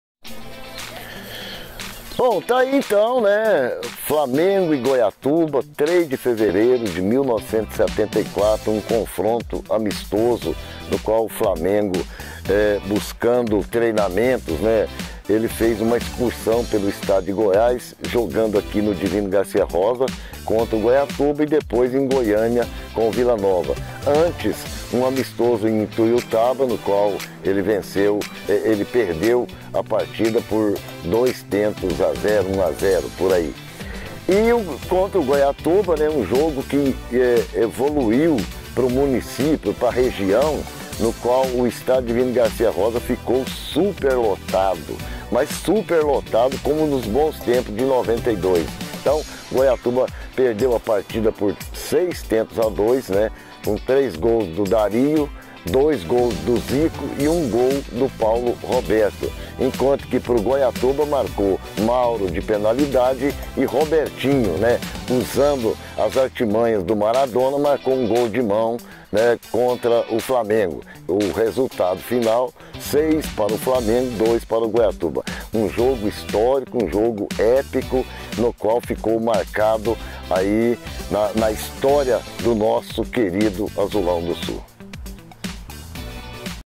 FLAMENGO VS GOIATUBA 74 POR NILTON VITURINO
O Historiador Nilton Viturino conta a História BDO Azulão em 1974⚽

Jogo 2681 - 3/2/1974 FLAMENGO 6X2 GOIATUBA (GO)

Competição: Amistoso Interestadual Local: Estádio Divino Garcia Rosa, Goiatuba (GO) Juiz: Benedito Gonçalves da Silva (GO) Flamengo: Renato (Cantarelli), Vanderlei, Chiquinho Pastor, Rondinelli (Ademir), Mineiro, Paulo Roberto (Afonsinho), Geraldo, Rogério (Paulinho Carioca), Dario, Zico, Arílson (Julinho) Gols: Dario (6, 68, 78), Zico (43,

59), Robertinho (74), Mauro (87), Paulo Roberto (90)